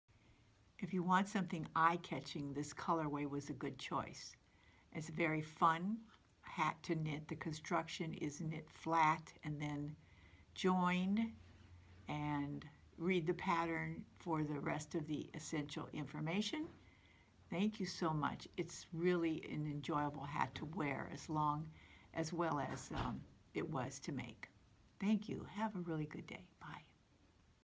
GOOD to wear - Bias Ridged Hat
A very fun pattern.  Knit flat.  A free pattern by rdtebby.  Using Red Heart Super Saver Stripes in Bright Stripe color way.